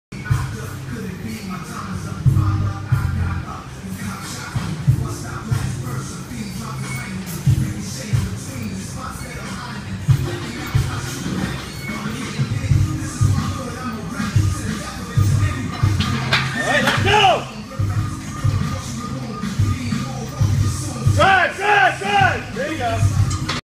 Aaron Palko 505x1 squat
Depth check for our final squat before the xpc semi finals